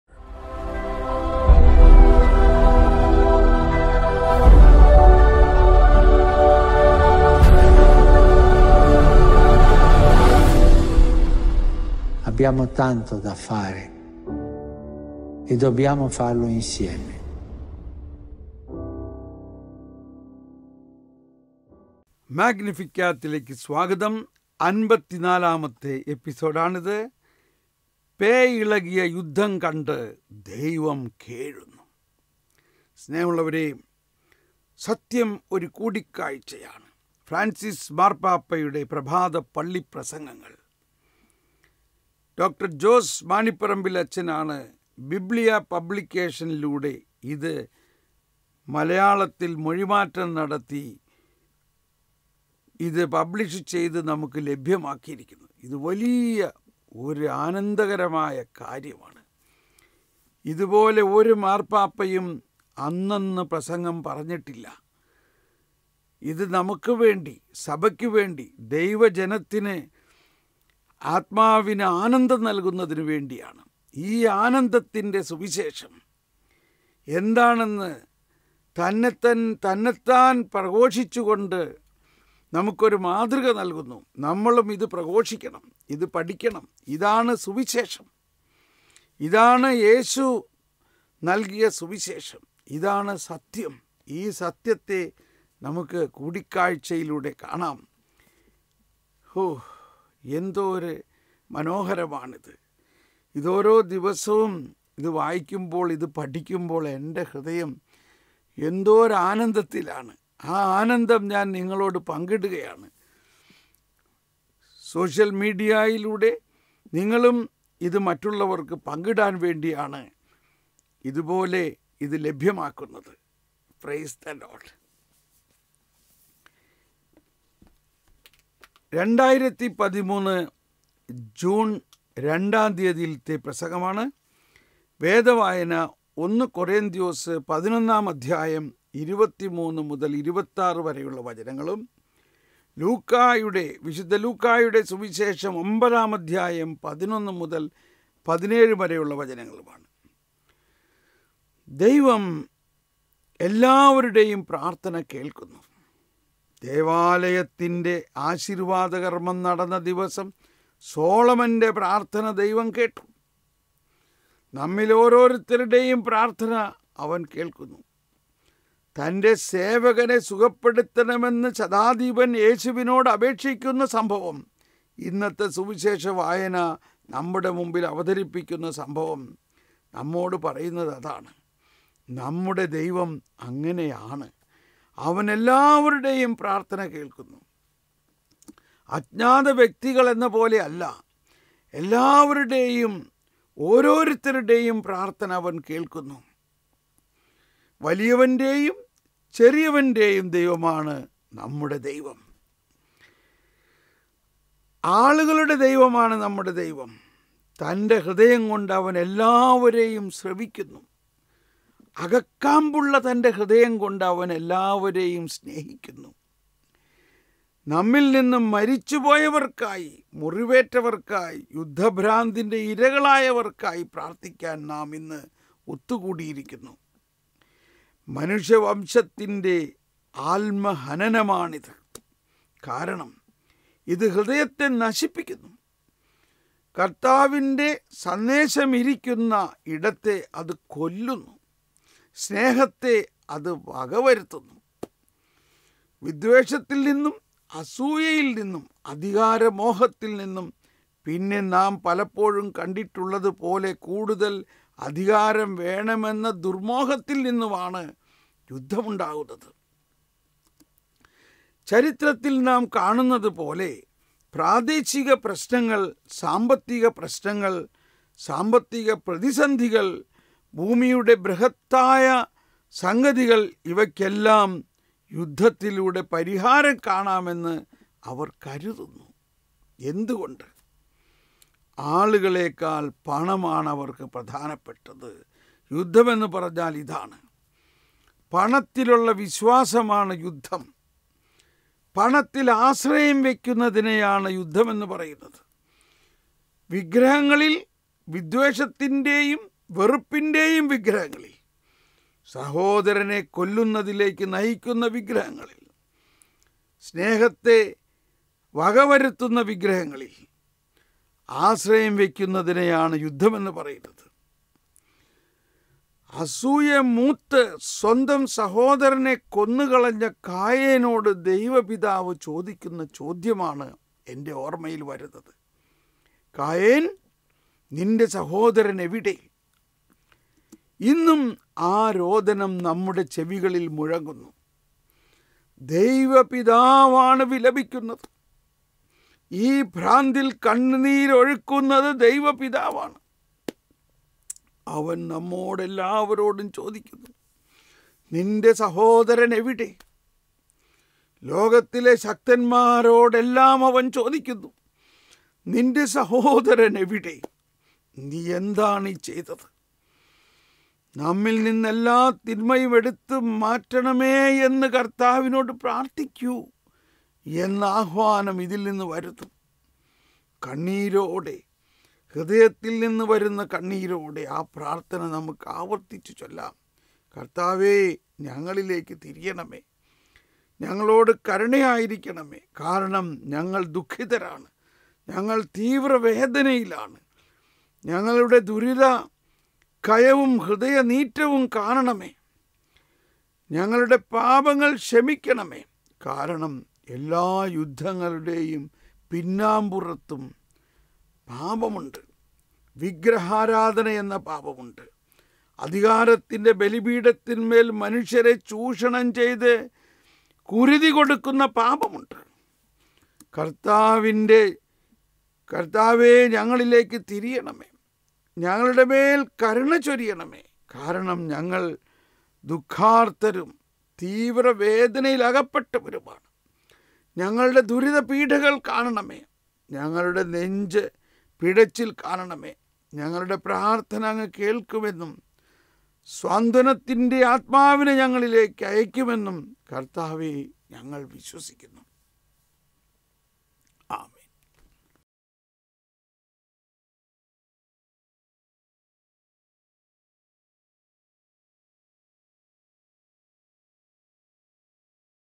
Homilies of Pope Francis || Br.Thomas Paul || പാപ്പായുടെ പള്ളി പ്രസംഗം - 54
പേ ഇളകിയ യുദ്ധം കണ്ടു. ദൈവം കേഴുന്നു (2 ജൂൺ 2013. വേദവായന: 1 കോറിന്തോസ് 11:23-26; ലുക്കാ 9:11-17)
ദൈവം എല്ലാവരുടെയും പ്രാർത്ഥന
കേൾക്കുന്നു . ദൈവാലയത്തിന്റെ ആശീർവ്വാദ
കർമ്മം നടന്ന ദിവസം സോളമന്റെ പ്രാർത്ഥന ദൈവം കേട്ടു. നമ്മിൽ ഓരോരുത്തരുടെയും പ്രാർത്ഥന അവൻ കേൾക്കുന്നു. തന്റെ സേവകനെ സുഖപ്പെടുത്തണമെന്നു ശതാധിപൻ യേശുവിനോട് അപേക്ഷിക്കുന്ന സംഭവം, ഇന്നത്തെ സുവിശേഷ വായന നമ്മുടെ മുമ്പിൽ അവതരിപ്പിക്കുന്ന സംഭവം, നമ്മോടു പറയുന്നത് അതാണ്. നമ്മുടെ ദൈവം അങ്ങനെയാണ്, അവൻ എല്ലാവരുടെയും പ്രാർത്ഥന കേൾക്കുന്നു. അജ്ഞാതവ്യക്തികൾ എന്നപോലെയല്ല, എല്ലാവരുടെയും, ഓരോരുത്തരുടെയും, പ്രാർത്ഥന അവൻ കേൾക്കുന്നു. വലിയവന്റെയും ചെറിയവ
ന്റെയും ദൈവമാണു നമ്മുടെ ദൈവം. ആളുകളുടെ ദൈവമാണു നമ്മുടെ ദൈവം. തന്റെ ഹൃദയംകൊണ്ട് അവൻ എല്ലാവരെയും ശ്രവിക്കുന്നു. അകക്കാമ്പുള്ള തന്റെ ഹൃദയംകൊണ്ട് അവൻ എല്ലാവരെയും സ്നേഹിക്കുന്നു.നമ്മിൽനിന്നു മരിച്ചു
പോയവർക്കായി, മുറിവേറ്റവർക്കായി, യുദ്ധഭ്രാന്തിന്റെ ഇരകളായവർക്കായി പ്രാർത്ഥിക്കാൻ നാം ഇന്ന് ഒത്തുകൂടി
യിരിക്കുന്നു. മനുഷ്യവംശത്തിന്റെ ആത്മഹനനമാണിത്. കാരണം, ഇതു ഹൃദയത്തെ നശിപ്പിക്കുന്നു. കർത്താവിന്റെ സന്ദേശമിരിക്കുന്ന ഇടത്തെ അതു കൊല്ലുന്നു. സ്നേഹത്തെ അതു വകവരുത്തുന്നു.
വിദ്വേഷത്തിൽ നിന്നും അസൂയയിൽനിന്നും അധികാരമോഹത്തിൽനിന്നും, പിന്നെ നാം പലപ്പോഴും കണ്ടിട്ടുള്ളതുപോലെ, കൂടുതൽ അധികാരം വേണമെന്ന ദുർമ്മോഹത്തിൽ നിന്നുമാണു യുദ്ധമുണ്ടാകുന്നത്.ചരിത്രത്തിൽ നാം കാണുന്നതുപോലെ, പ്രാദേശിക പ്രശ്നങ്ങൾ, സാമ്പത്തിക പ്രശ്നങ്ങൾ, സാമ്പത്തിക പ്രതിസന്ധികൾ, ഭൂമിയുടെ ബൃഹത്തായസംഗതികൾ ഇവയ്ക്കെല്ലാം യുദ്ധത്തിലൂടെ പരിഹാരം കാണാം എന്ന് അവർ കരുതുന്നു.എന്തുകൊണ്ട്? ആളുകളേ ക്കാൾ  പണമാണ് അവർക്കു പ്രധാനപ്പെട്ടത് യുദ്ധമെന്നു പറഞ്ഞാൽ ഇതാണ്:
പണത്തിലുള്ള വിശ്വാസമാണു യുദ്ധം, പണത്തിൽ ആശ്രയം വയ്ക്കുന്നതിനെയാണു യുദ്ധമെന്നു പറയുന്നത്. വിഗ്രഹങ്ങളിൽ, വിദ്വേഷത്തിന്റെയും വെറുപ്പിന്റെയും വിഗ്രഹങ്ങളിൽ, സഹോദരനെ കൊല്ലുന്നതിലേക്കു നയിക്കുന്ന വിഗ്രഹങ്ങളിൽ
സ്നേഹത്തെ വകവരുത്തുന്ന വിഗ്രഹങ്ങളിൽ ആശ്രയംവയ്ക്കുന്നതിനെയാണു യുദ്ധമെന്നു പറയുന്നത്. അസൂയ മൂത്തു സ്വന്തം
സഹോദരനെ കൊന്നുകളഞ്ഞ കായേനോടു ദൈവപിതാവു ചോദിക്കുന്ന ചോദ്യമാണ് എന്റെ ഓർമ്മയിൽ വരുന്നത്. കായേൻ,
നിന്റെ സഹോദരനെവിടെ? ഇന്നും ആരോദനം നമ്മുടെ ചെവികളിൽ മുഴങ്ങുന്നു. ദൈവപിതാവാണു വിലപിക്കുന്നത്. ഈ ഭ്രാന്തിൽ കണ്ണീരൊഴുക്കുന്നതു ദൈവ
പിതാവാണ്. അവൻ നമ്മോടെല്ലാവരോടും ചോദിക്കുന്നു: നിന്റെ സഹോദരൻ എവിടെ? ലോകത്തിലെ ശക്തന്മാരോടെല്ലാം അവൻ ചോദിക്കുന്നു നിന്റെ സഹോദരൻ എവിടെ? നീ എന്താണ് ഈ ചെയ്തത്?നമ്മിൽനിന്ന് എല്ലാ തിന്മയും എടുത്തുമാറ്റണമേയെന്നു കർത്താവിനോടുപ്രാർത്ഥിക്കൂ എന്ന ആഹ്വാനം ഇതിൽനിന്നു വരുന്നു.
കണ്ണീരോടെ, ഹൃദയത്തിൽനിന്നു വരുന്ന കണ്ണീരോടെ, ആ പ്രാർത്ഥന നമുക്ക് ആവർത്തിച്ചുചൊല്ലാം.കർത്താവേ, ഞങ്ങളിലേക്കു തിരിയണമേ; ഞങ്ങളോടു കരുണയായിരിക്കണമേ, കാരണം, ഞങ്ങൾ ദുഃഖിതരാണ്. ഞങ്ങൾ തീവ്രവേദനയിലാണ്
ഞങ്ങളുട ദുരിതക്കയവും ഹൃദയനീറ്റലും കാണണമേ. ഞങ്ങളുടെ പാപ്ങ്ങൾ
ക്ഷമിക്കണമേ. കാരണം, എല്ലാ യുദ്ധങ്ങളുടെ പിന്നാമ്പുറത്തും പാപമുണ്ട്. വിഗ്രഹാരാധന
യെന്ന പാപമുണ്ട്. അധികാരത്തിന്റെ ബലിപീഠത്തിൻമേൽ മനുഷ്യരെ ചൂഷണം ചെയ്ത കുരുതി കൊടുക്കുന്ന പാപമുണ്ട്. കർത്താവേ, ഞങ്ങളിലേക്കു തിരിയണമേ. ഞങ്ങളുടെമേൽ കരുണ ചൊരിയണമേ. കാരണം, ഞങ്ങൾ ദുഃഖാർത്തരും തീവ്ര
വേദനയിലകപ്പെട്ടവരുമാണ്. ഞങ്ങളുടെ ദുരിതപീഡകൾ കാണണമേ. ഞങ്ങളുടെ നെഞ്ചുപിടചിൽ കാണണമേ. ഞങ്ങളുടെ പ്രാർത്ഥന അങ്ങു കേൾക്കുമെന്നും സാന്ത്വനത്തിന്റെ ആത്മാവിനെ ഞങ്ങളിലേക്ക് അയക്കുമെന്നും, കർത്താവേ, ഞങ്ങൾ വിശ്വസിക്കുന്നു.